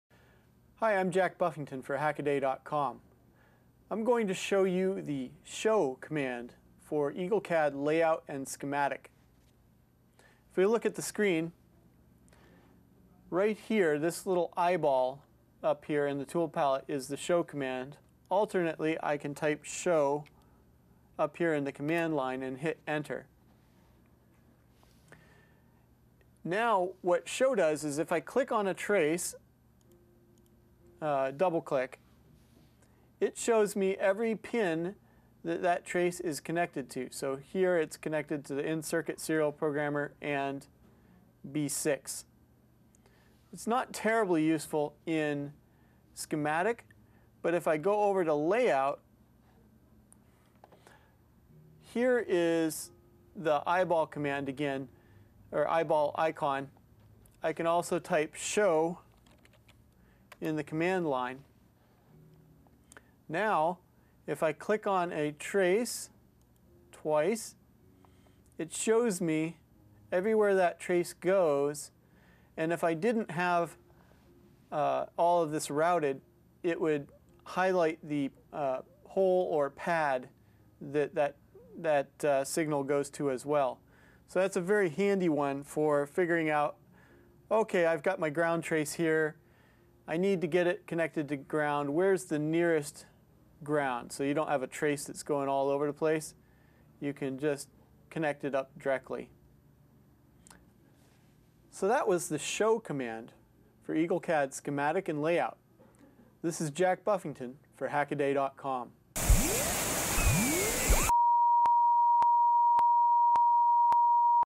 Eagle CAD - show command
In this Hackaday.com original video, [Jack] shows Eagle CAD's 'show' command.